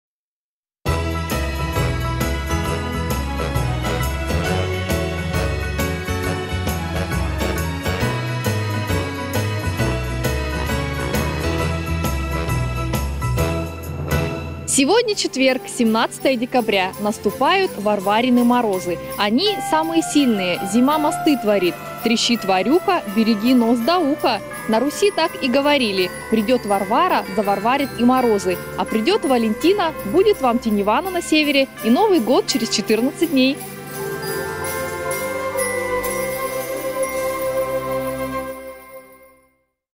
Народный календарь от корреспондентов «Севера»: 17 декабря
Сегодня четверг, 17 декабря.  Наступают Варварины морозы, они самые сильные. Зима мосты творит, трещит Варюха — береги нос да ухо. На Руси так и говорили, погоди, придет Варвара, заварварят и морозы. А придет Валентина, будет вам «Теневана» на «Севере». И Новый год через 14 дней!